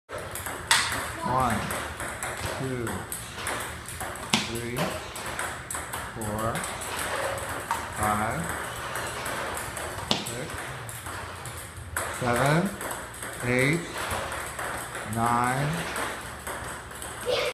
Spinny Serve Return Challenge
Jason Piech achieved Samson Dubina’s challenge of hitting 10 balls in a row.